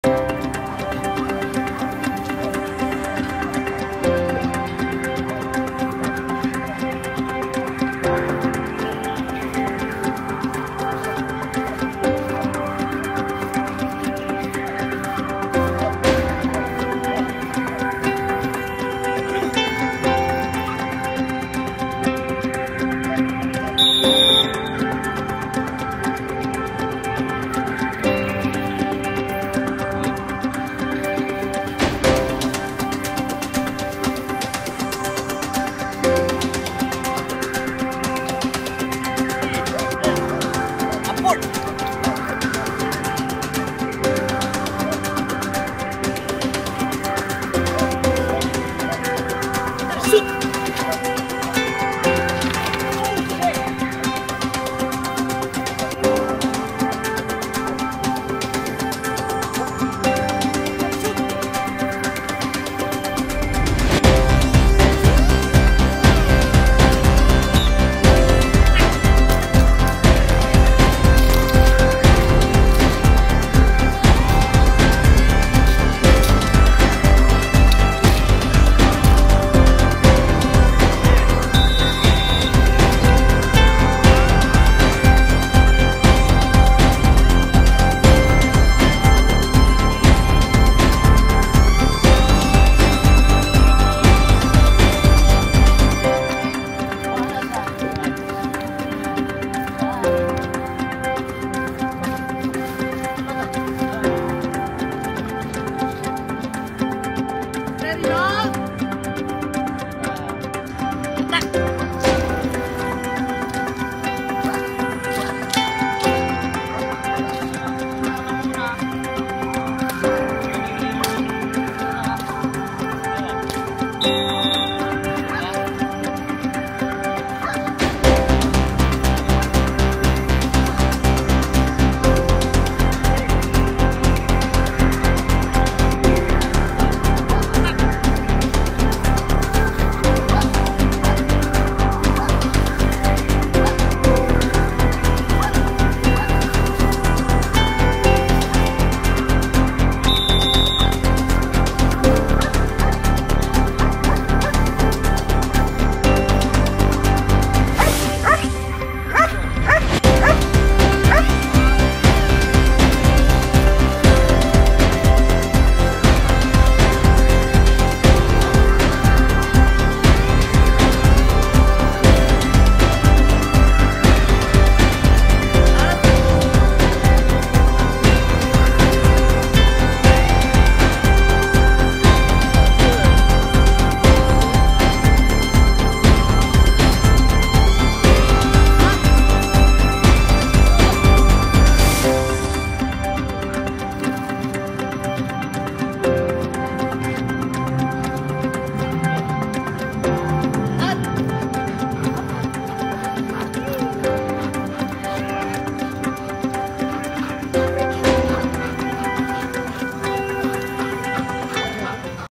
MONDIORING TRAINING: Tera Laroja (BSD)
Tera Laroja (Terry)
Belgian Malinois
Training for Mondioring Category 2
Philippine Mondioring Association Inc.